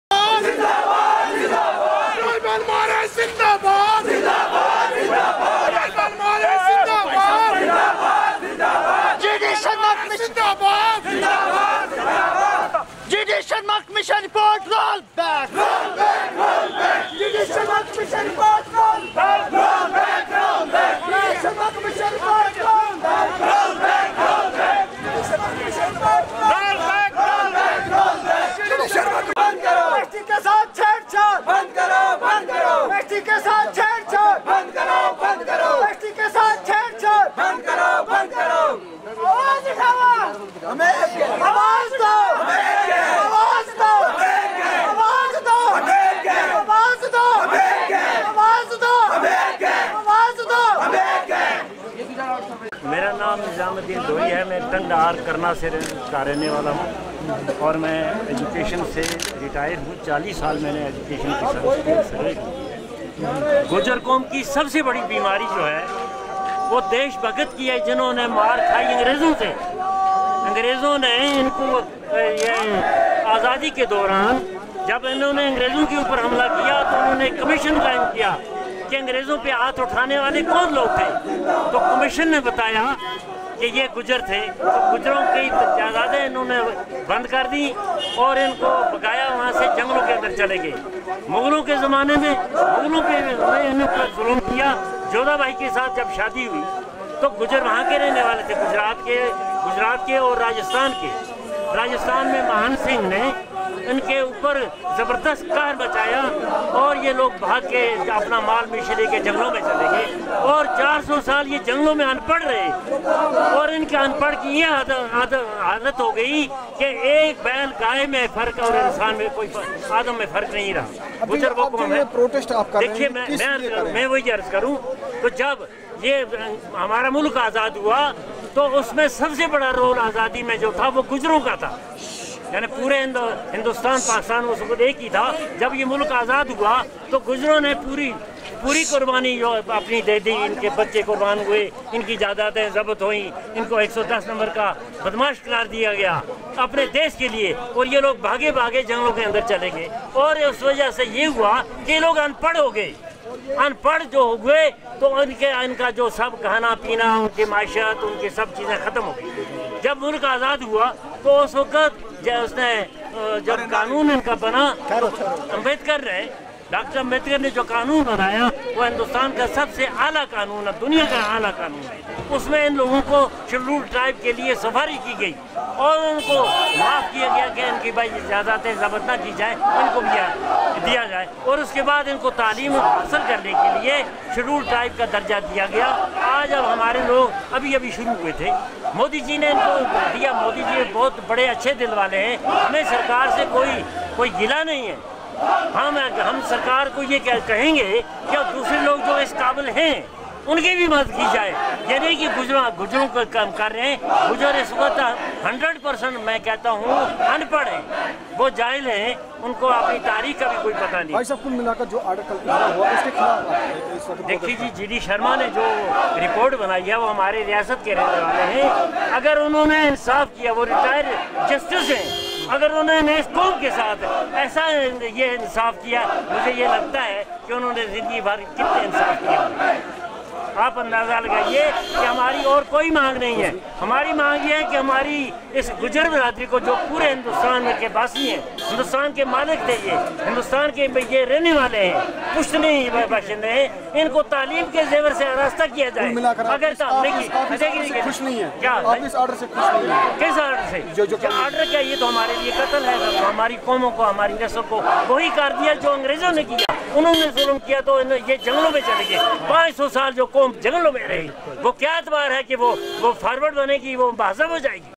شہر سرینگر کے لالچوک میں آج گوجر بکروال کمنیٹی کے لوگوں نے جی ڈی شرما کے آڈر پرایک زو دار احتجاج کیا
شہر سرینگر کے لالچوک میں آج گوجر بکروال کمنیٹی کے لوگوں نے جی ڈی شرما کے آڈر پر ایک زور دار احتجاج کیا